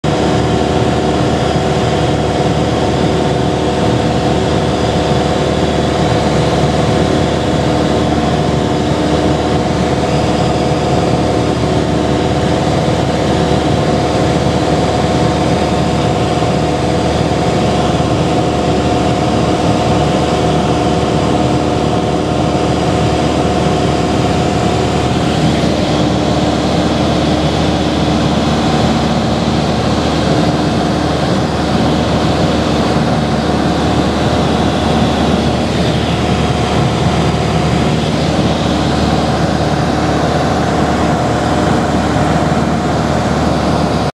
Over the Verrazano Bridge
Flying the Hudson River in Cessna 177 Cardinal N177SM from Harford County Airport 0W3